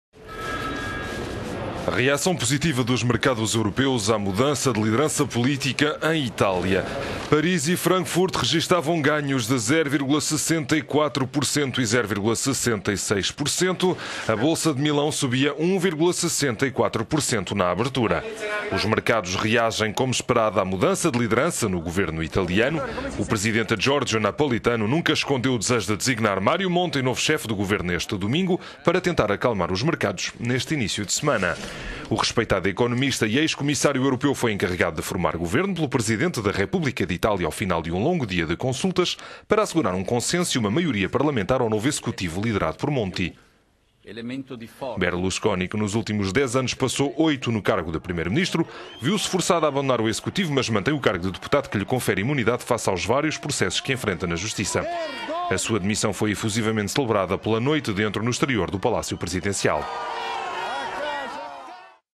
Mercados invertem tendência da abertura e seguem no vermelho
A reação positiva dos mercados europeus à mudança de liderança política em Itália não durou muito tempo.

As bolsas de Paris e Frankfurt registaram ganhos superiores a meio por cento no início da sessão. Milão subia 1,64%. No entanto uma hora e meia depois os mercados inverteram a tendência e negociavam no vermelho.

O presidente Giorgio Napolitano nunca escondeu o desejo de designar Mario Monti novo chefe de governo este domingo para tentar acalmar os mercados neste início de semana.